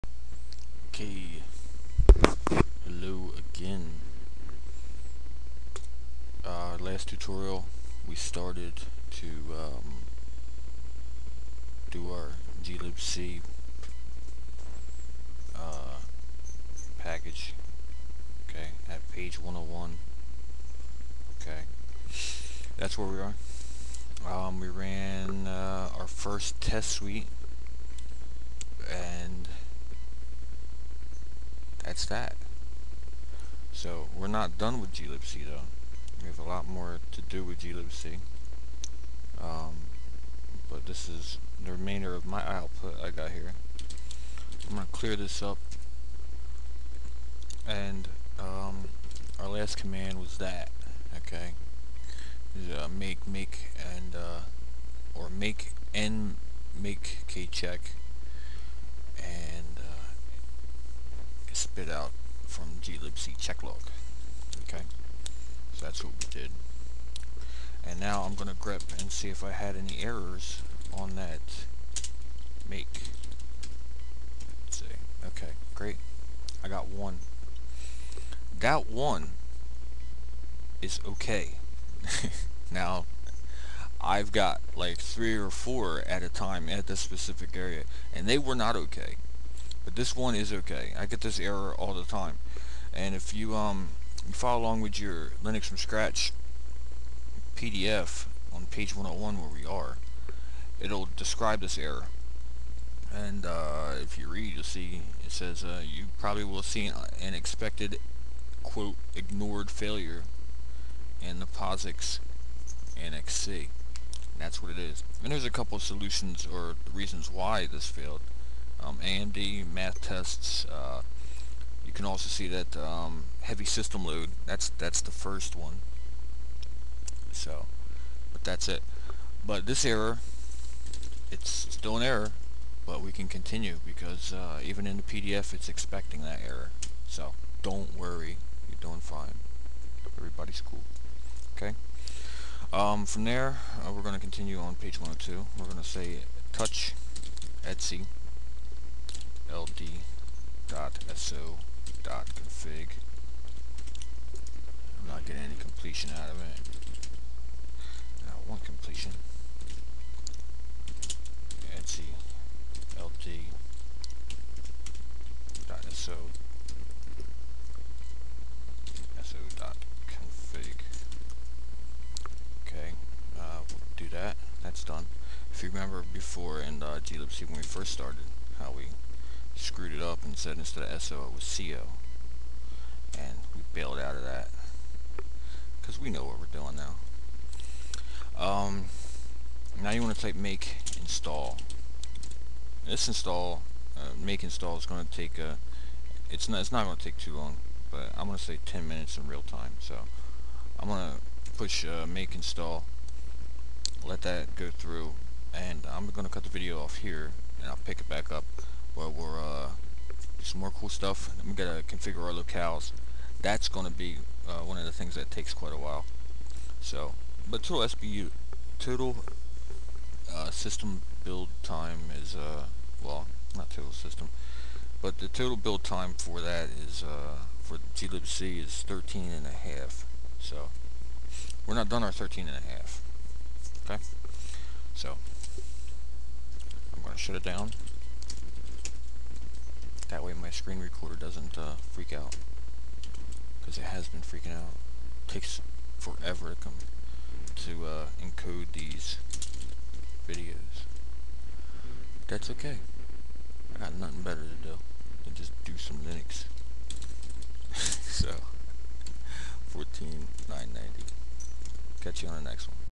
complete linux from scratch install part20of31
this is only good for lfs 6.2 x86 heres a link.
and get the lfs 6.2 book. if you have a different architecture these vids may not touch on everything you need to know but theyll help. Read The Book